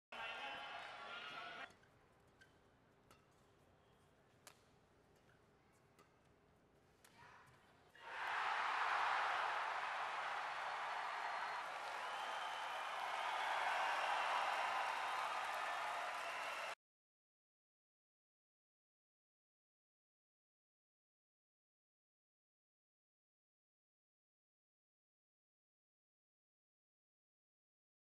LCW Bags Fifth BWF Best Male Player Of The Year Award
Sports, badminton.

The country's ace shuttler, Datuk Lee Chong Wei claimed his fifth Badminton World Federation, BWF Best Male Player of the Year Award at the Players' Reception and Gala Dinner in Dubai, United Arab Emirates.

Datuk Lee sidelined other nominees including compatriots and Olympics men's doubles silver medallist pair, Goh V Shem-Tan Wee Kiong, China's Olympics men's singles gold medallist Chen Long and men's doubles pair, Fu Haifeng-Zhang Nan, and Denmark's Jan O Jorgensen.

In the 2016 season, the top national shuttler had bagged three World Superseries titles in Malaysia, Indonesia and Japan and his third Olympics silver medal at Rio in August, while going for his fifth world superseries finals title in Dubai.

The 34-year-old Penangite had received the honour in 2009, 2010, 2011 and 2013.